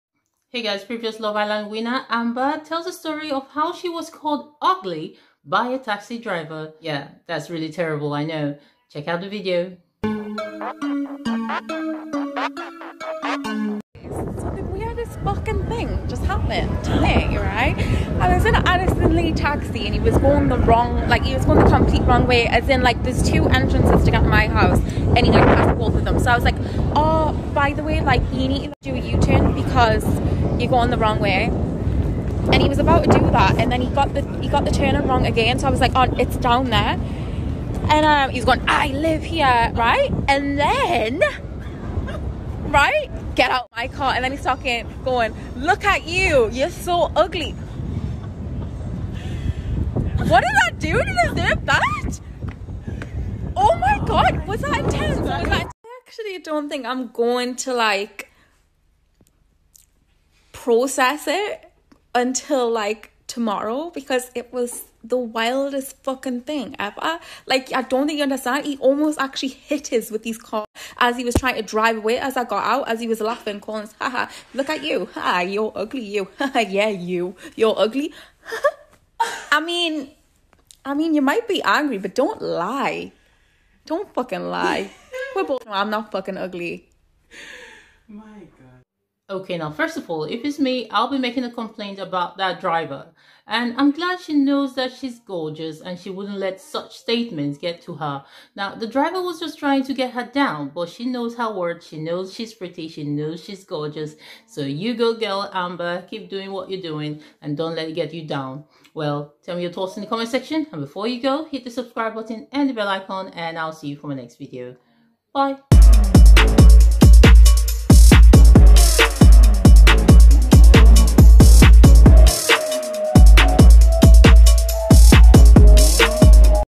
Previous Love Island Winner Amber Rose Gill In Shock As Taxi Driver Called Her UGLY
Love Island Amber Rose Gill